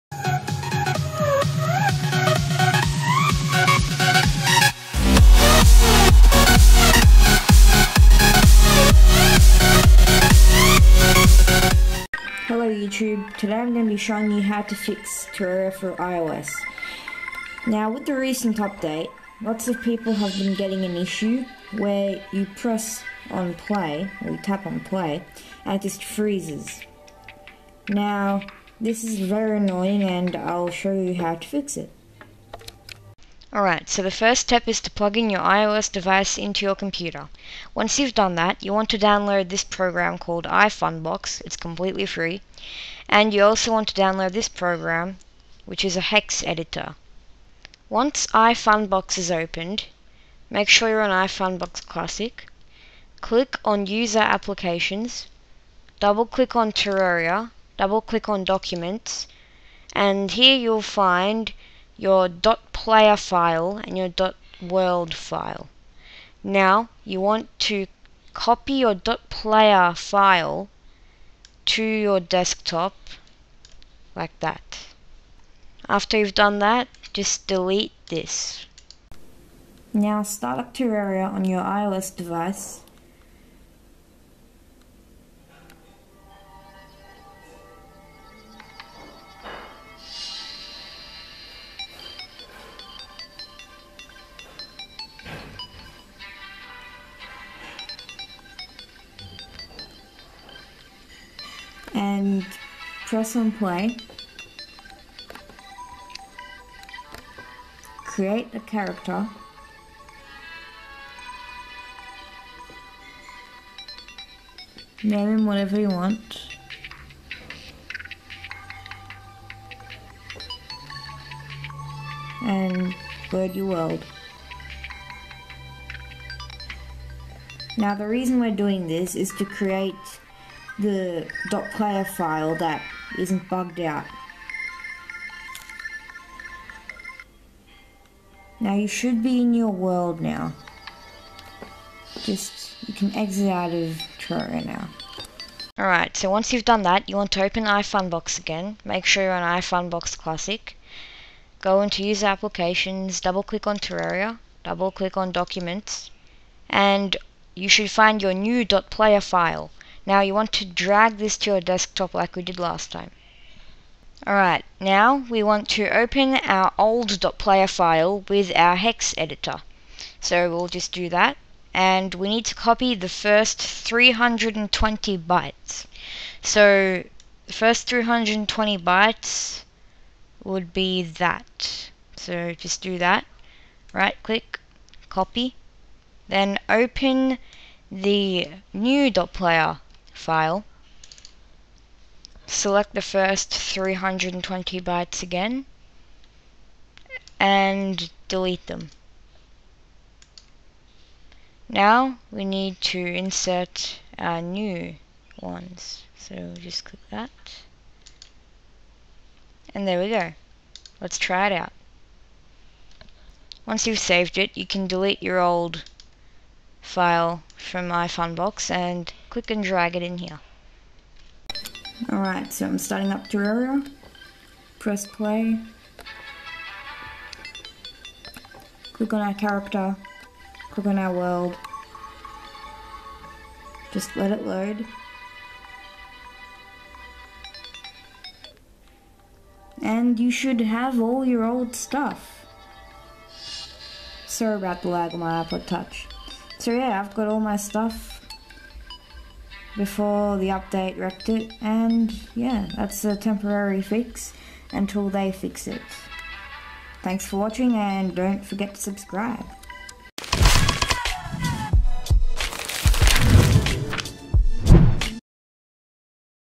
How to fix iOS Terraria freezing on play
This will NOT remove your world/save files.